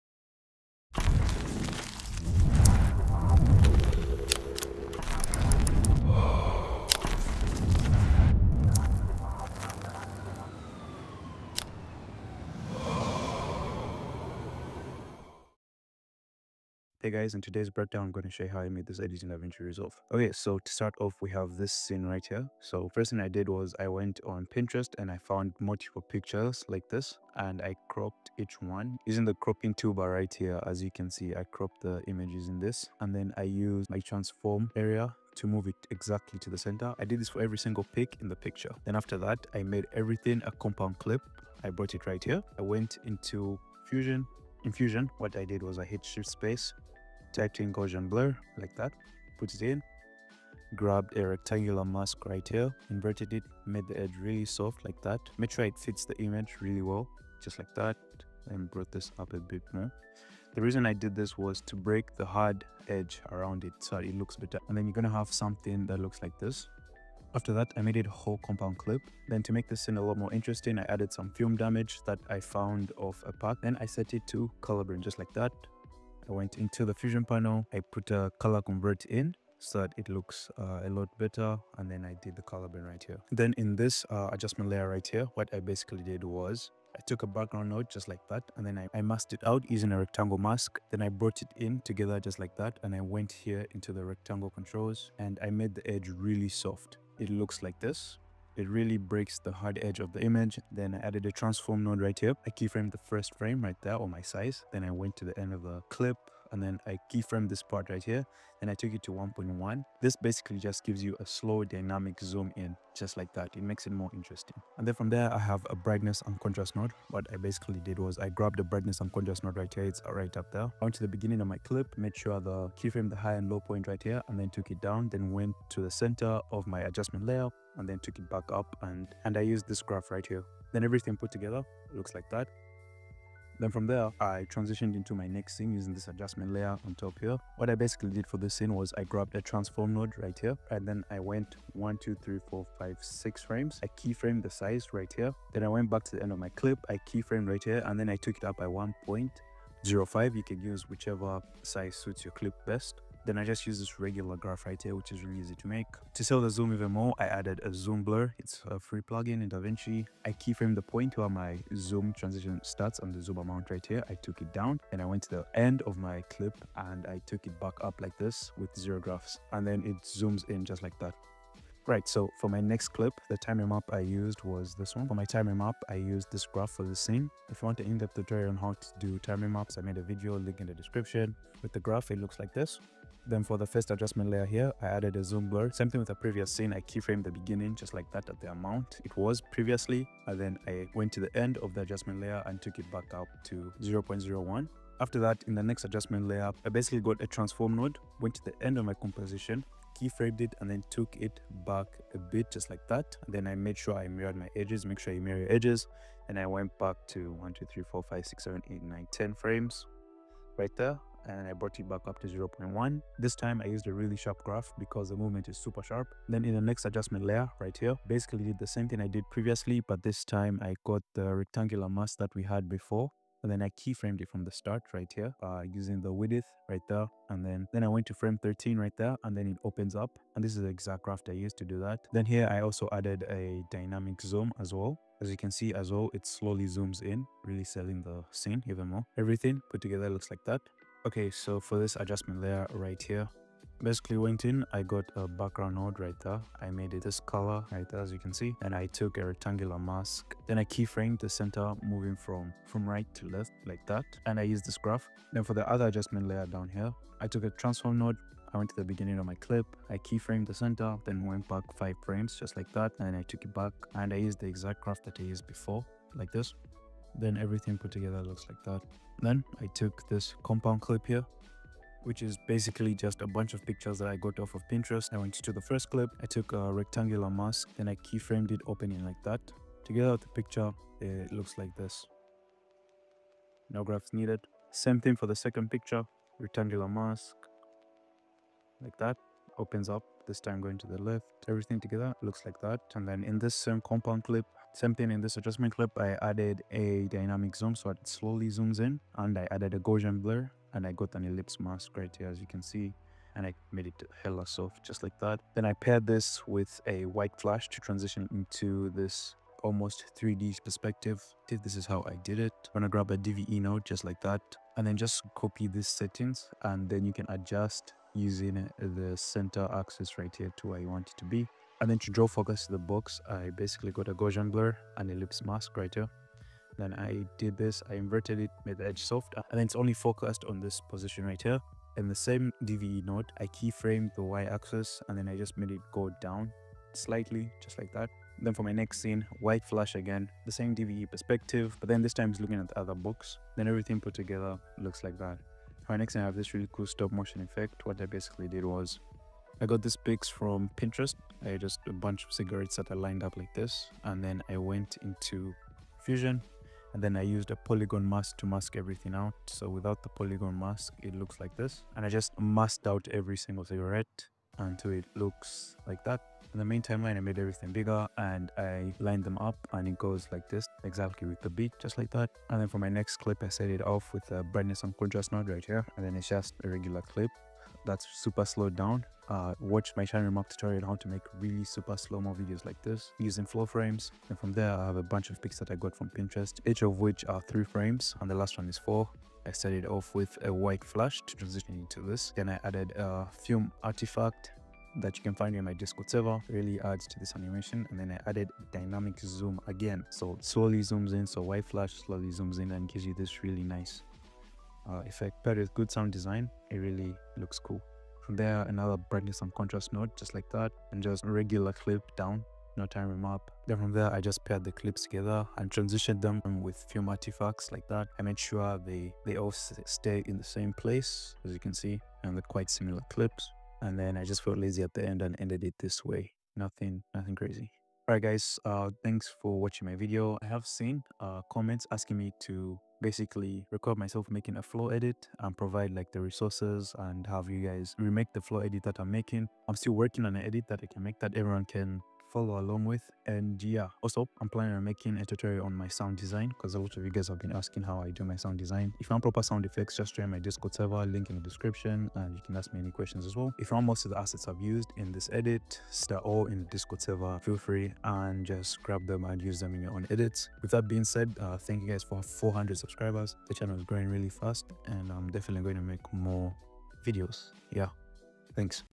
DaVinci Resolve | Flow Edit Tutorial Breakdown Pt 3
In today's video, I'm breaking down my full DaVinci Resolve flow edit. I'll show you how I make flow, the nodes I use, how I structure my effects, and all the resources that help me get clean and smooth motion. If you've been trying to perfect your flow edits, this video will guide you through everything step by step.